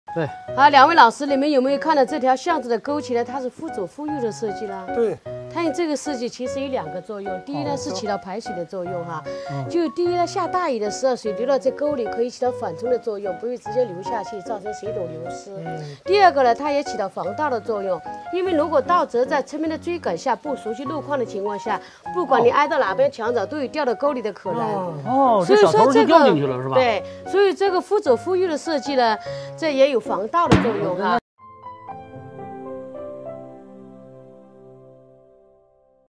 Can ditches prevent thieves ?
We probably all know that the purpose of the ditch is to drain the water on the ground when it rains, but some of the ditches are on the left side of the alleyway and some are on the right side of the alleyway. Why is this?